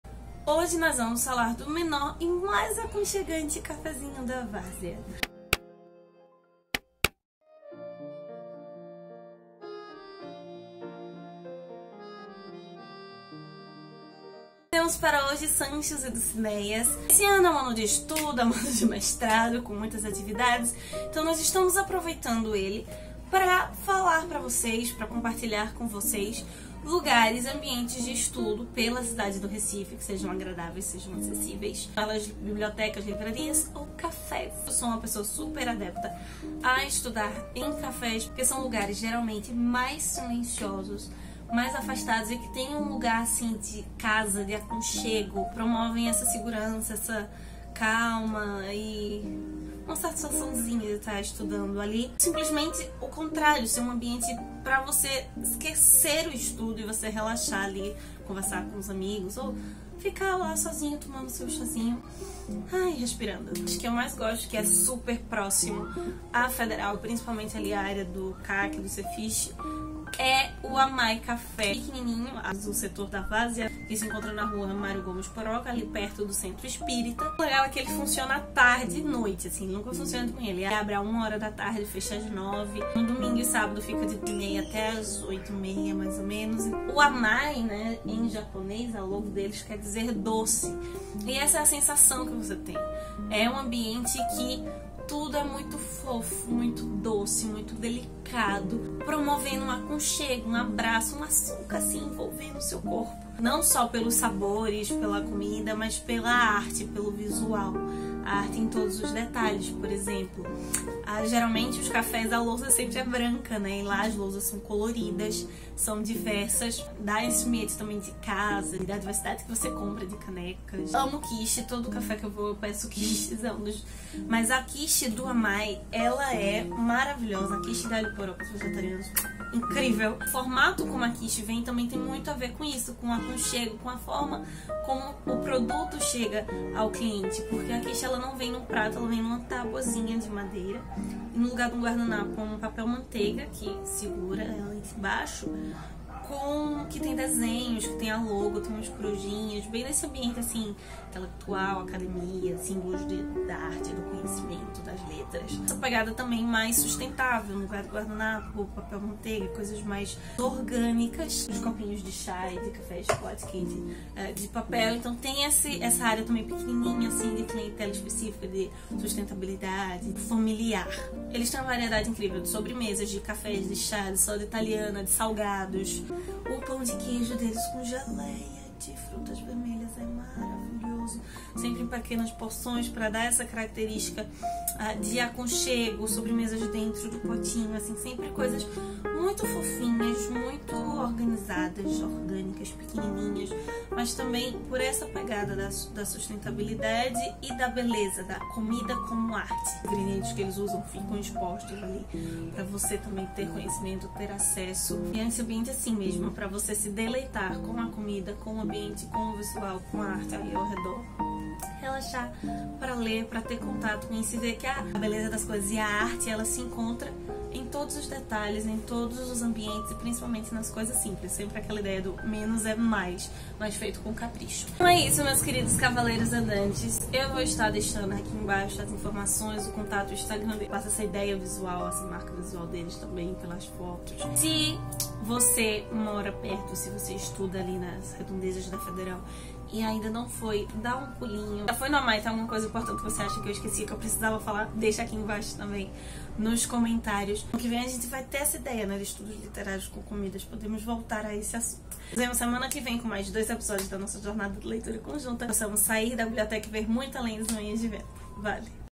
VLOG#57 - Amai Café
Para quem gosta e precisa sair de casa para estudar em um lugar calmo e agradável, nada melhor do que um unir isto a um cházinho e proximidade da universidade. Se você estuda na UFPE, campus Recife, claro. Hoje compartilho com vocês um dos meus café favoritos da Várzea  - não só pelas comidinhas mas pela beleza estética do lugar e da proposta - e o mais próximo a área de humanas.